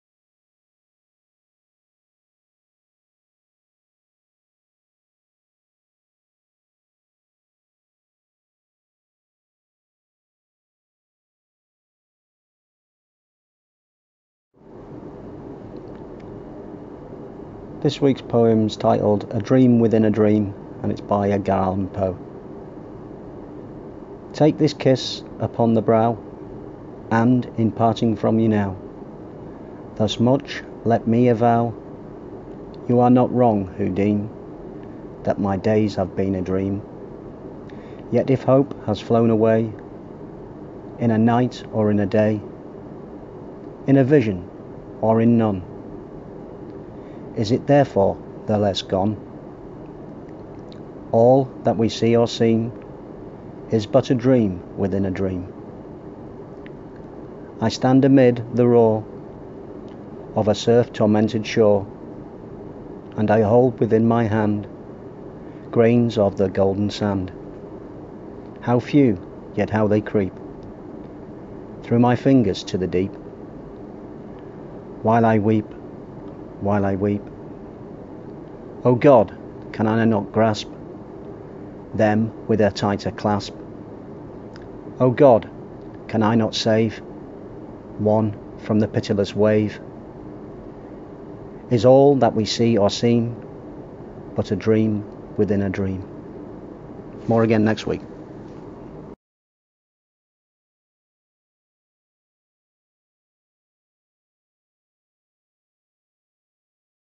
"A Dream within a dream" by Edgar Allan Poe
A reading by Zak Hardacre